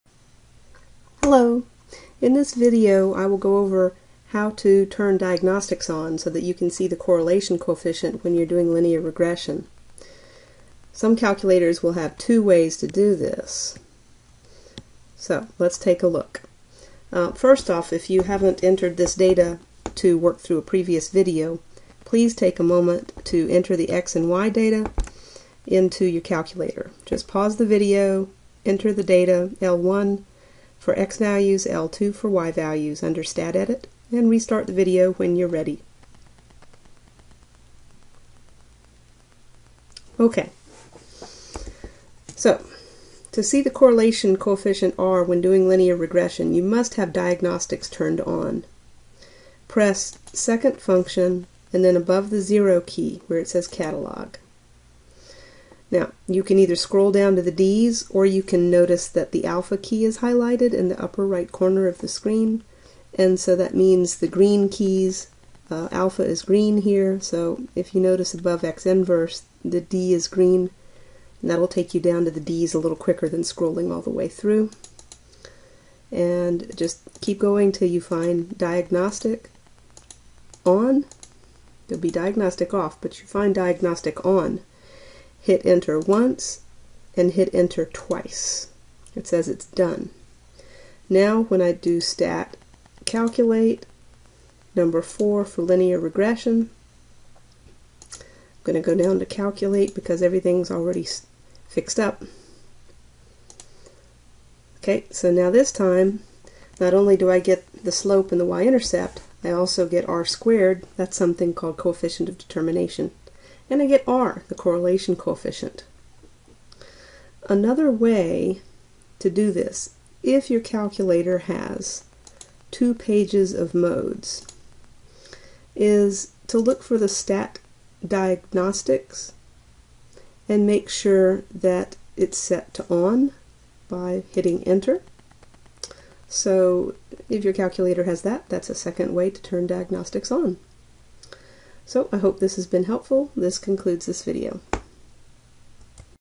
Correlation Coeffcient Diagnostics ON
To display the correlation coefficient and coefficient of determination when doing linear regression, turn diagnostics ON. For a .pdf copy of the annotated handout seen in the video, click the following